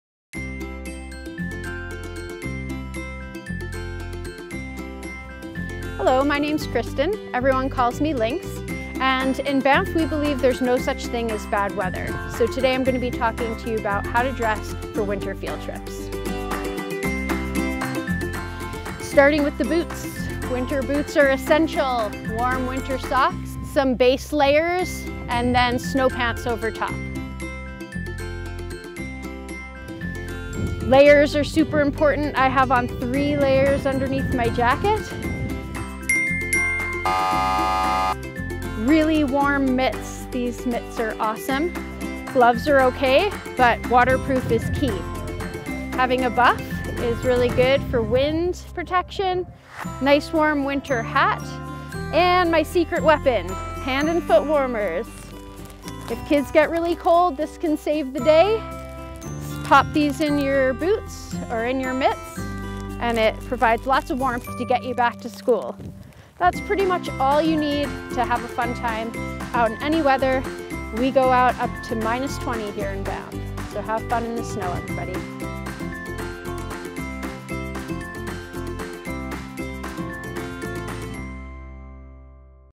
Outdoor Learning - What to Wear for Winter
Lynx shares a brief description of what kind of clothing and layering should be considered for taking students outside.